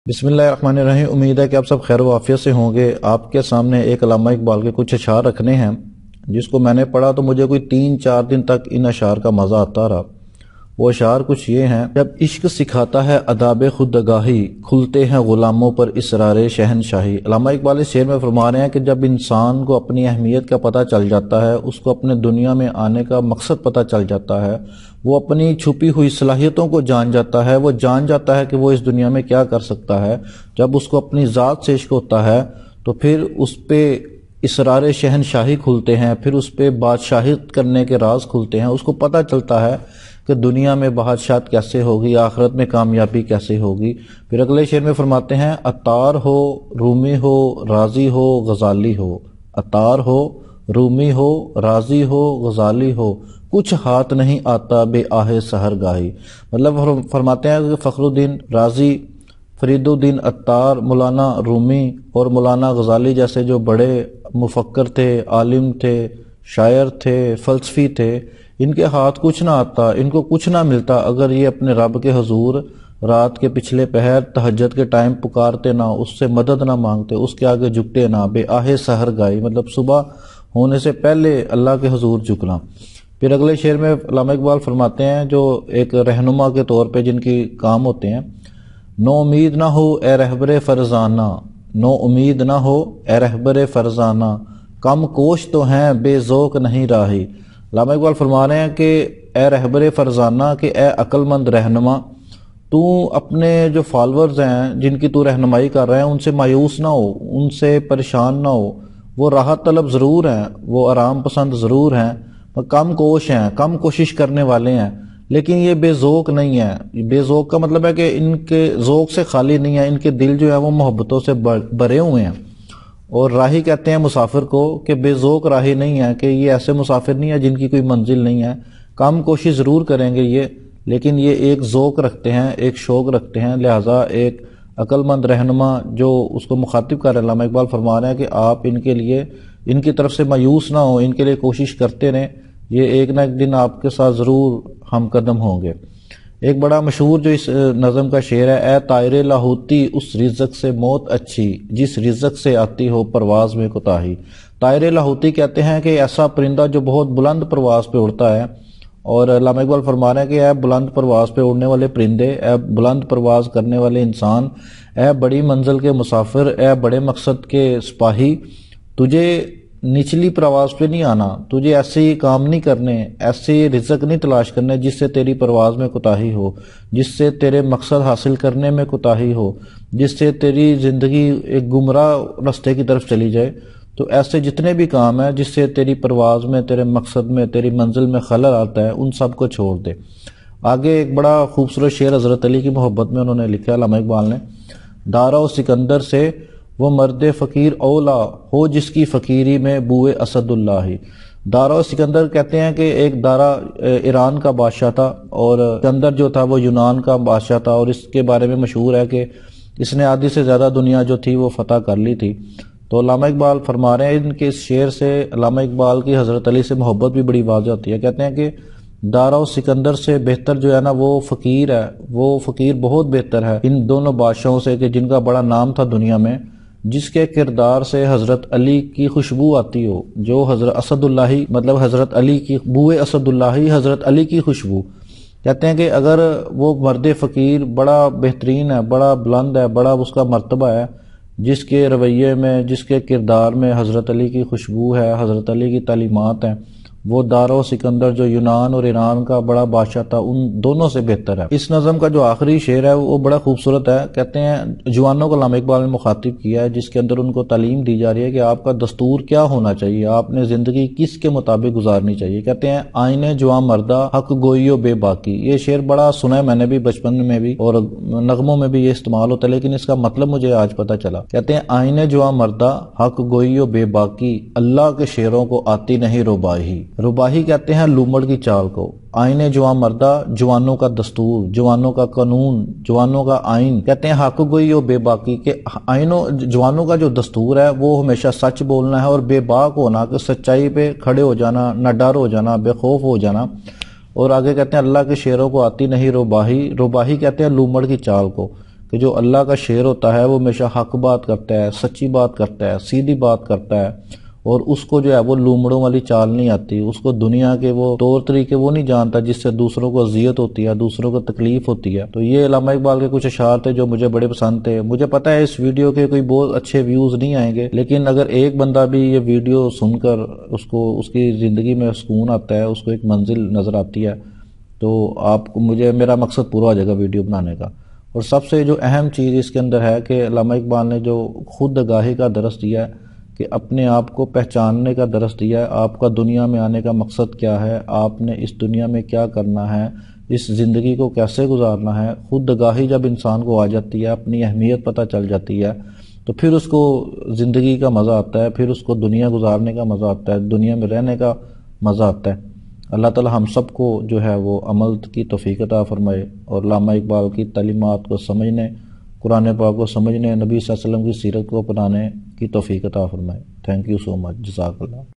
Poetry of Iqbal | Kalam e Iqbal : Jb ishq sikhata ha adab e khud aghai poetry
This video is all about the inspirational poems written by Allama Iqbal and Kalam e Iqbal. Learn about the amazing impact these pieces of literature have had on generations of people and get inspired by the incredible words of wisdom that Iqbal and Kalam e Iqbal have left behind. Get ready to explore the beauty of these works of art, and find inspiration in the verses alive in them!

Kalam e Iqbal : Jb ishq sikhata ha adab e khud aghai
allama iqbal poetry and details tashreeh by Haider ali| Allama IQBAL
allama iqbal about youth